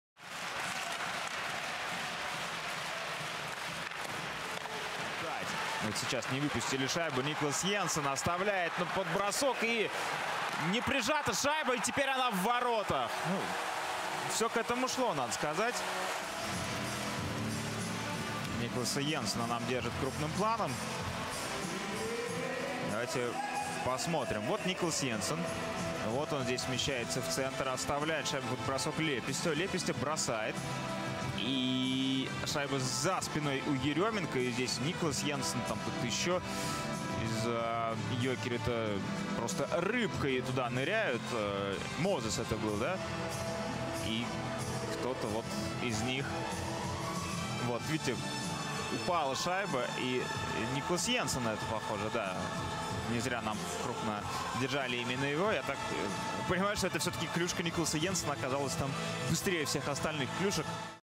Енсен добивает шайбу в ворота
Никлас Енсен увидел шайбу за спиной у Еременко и успел отправить ее в ворота